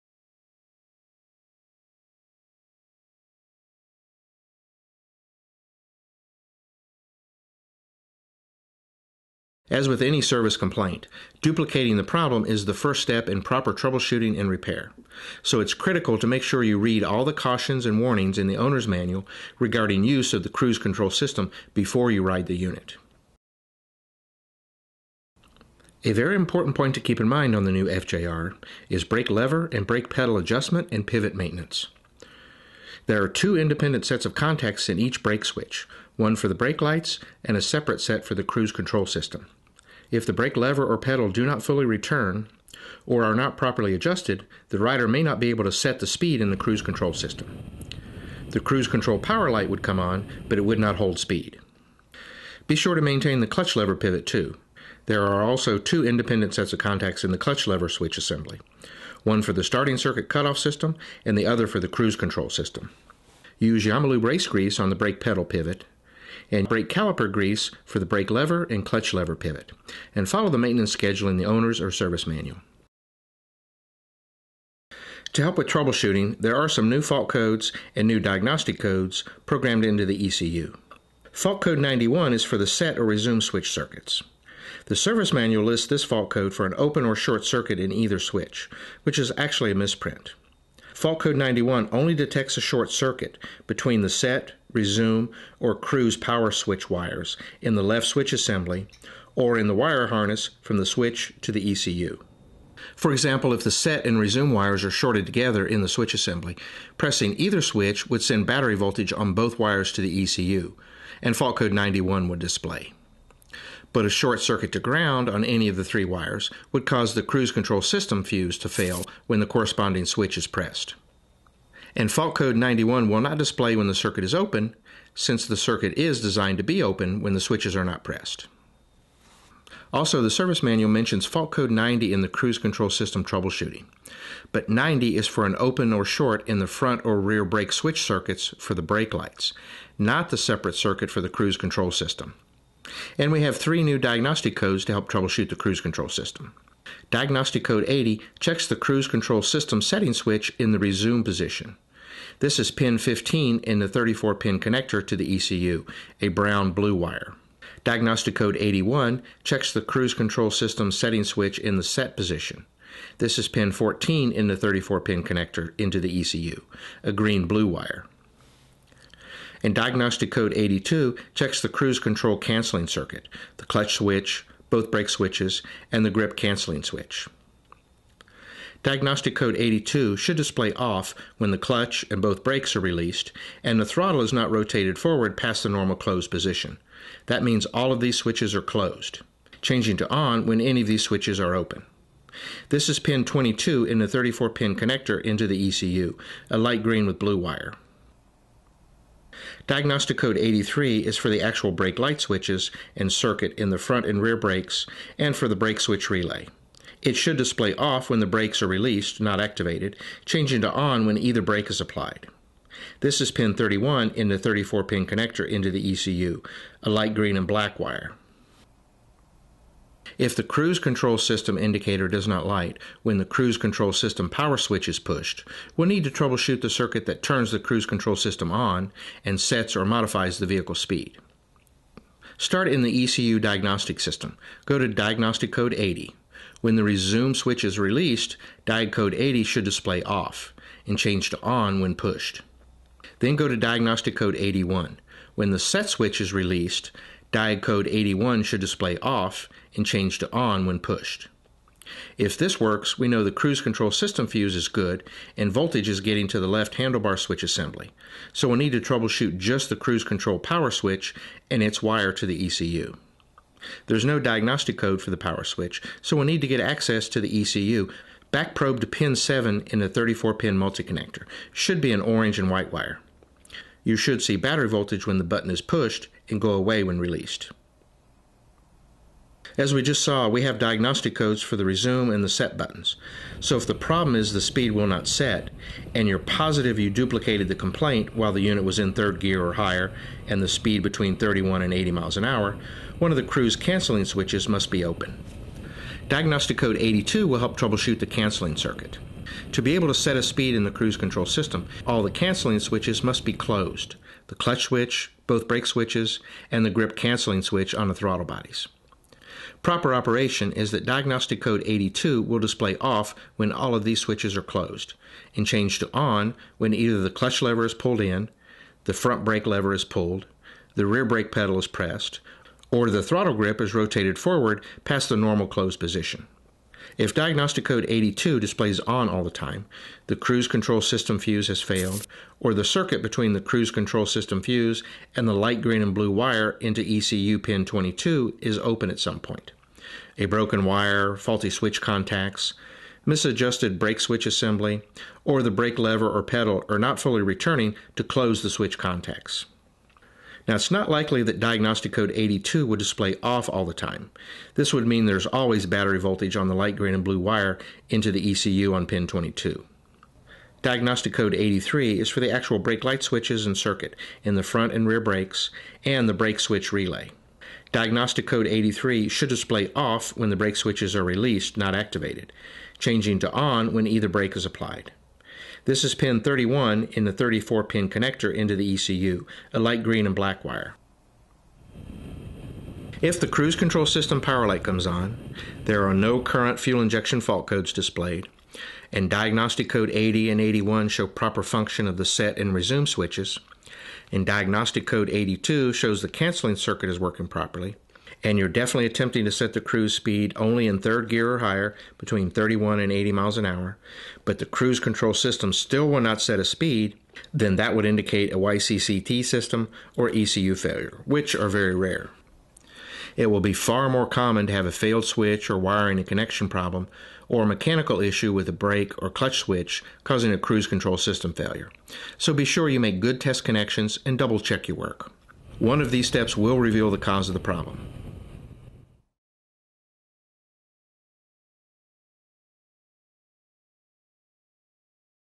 Yamaha Cruise Control System Troubleshooting
Troubleshooting tips for the new Cruise Control System used on the 2013 Yamaha FJR1300A.
3:39 Cruise Control will not power ON